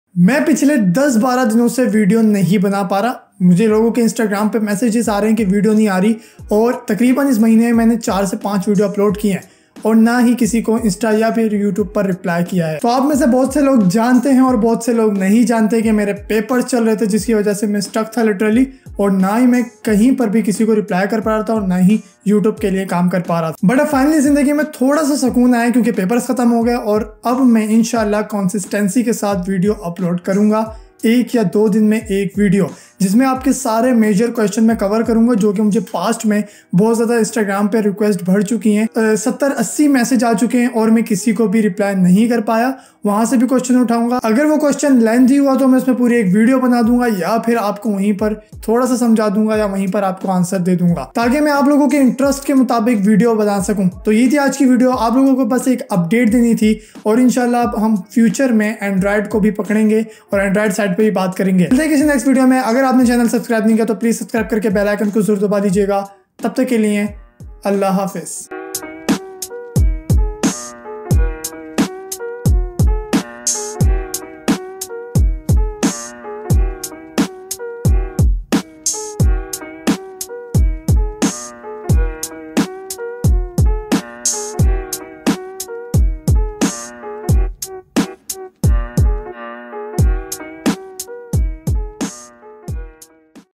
ME VIDEO UPLOAD KYUN NAHI KAR RAHA?
So in this video, I tell you why I'm not uploading videos on youtube :(